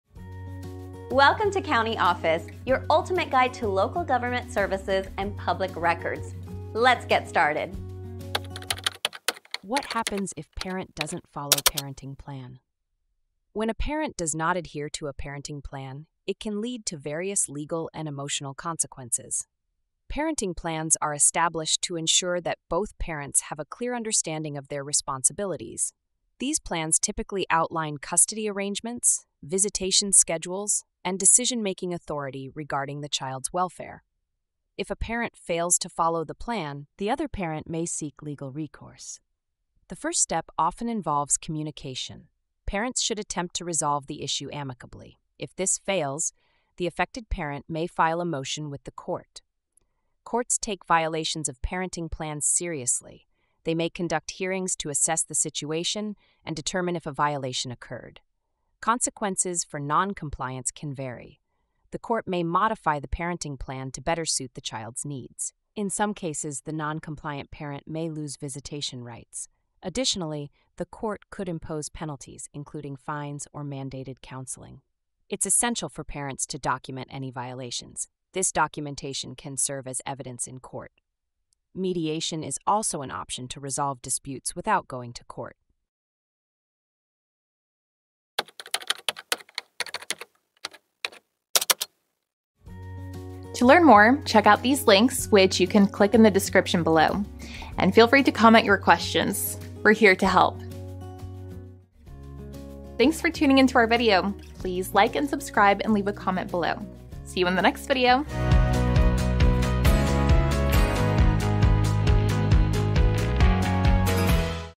What Happens If Parent Doesn't Follow Parenting Plan? - CountyOffice.org
What Happens If Parent Doesn't Follow Parenting Plan? Are you dealing with a parent who isn't following the agreed-upon parenting plan? Discover the legal and emotional consequences in our latest video. Learn about the importance of parenting plans, custody arrangements, and visitation schedules for the welfare of the child. Find out what steps to take if a parent violates the plan, from communication to seeking legal recourse through court intervention. Understand the potential outcomes for non-compliance, including modifications to the plan and possible loss of visitation rights.

Documenting violations is crucial, and mediation can be a helpful tool in resolving disputes. Stay informed and empowered when facing parenting plan challenges.